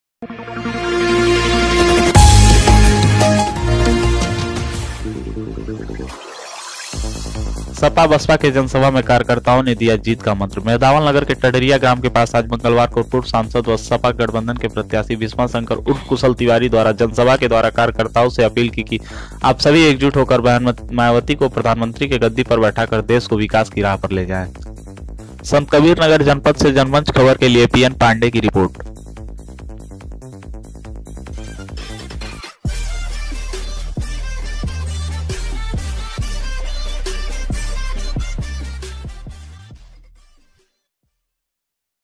सपा - बसपा की रैली में कार्यकर्ताओं को दिया गया जीत का मंत्र- संतकबीरनगर नगर
संतकबीरनगर जनपद में बसपा के कार्यकर्ताओं की रैली संपन्न हुई । जिसमें बहन मायावती को प्रधानमंत्री बनाने के लिए संकल्प लिया गया।

संतकबीरनगर से पीएम पांडे की रिपोर्ट

U can also send your news and script with your number and complete details .